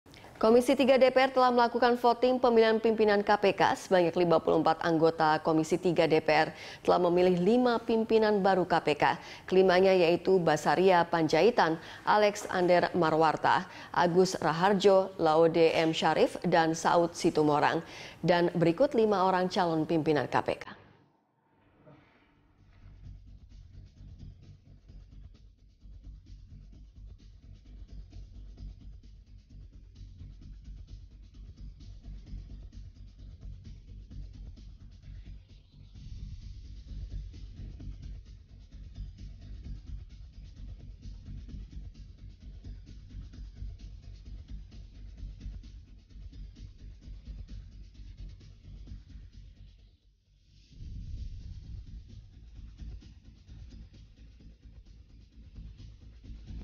5 Wajah Baru Pimpinan KPK
Sebanyak 54 anggota Komisi III telah memilih lima pimpinan baru kpk. Kelimanya yaitu Basaria Panjaitan, Alexander Marwarta, Agus Rahardjo, Laode M Syarif, dan Saut Situmorang.

Facebook.com/BeritaSatuTV
Youtube.com/BeritaSatu